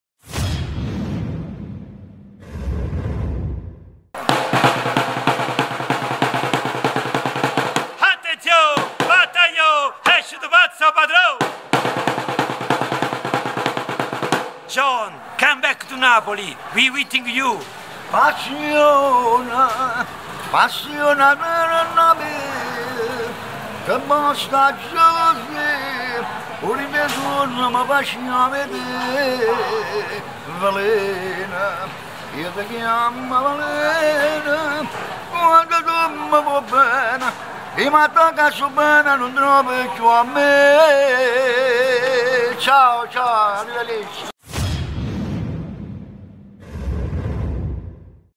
Per John Turturro da 'O Pazzariello
Il saluto di 'O Pazzariello (Angelo Picone) per John Turturro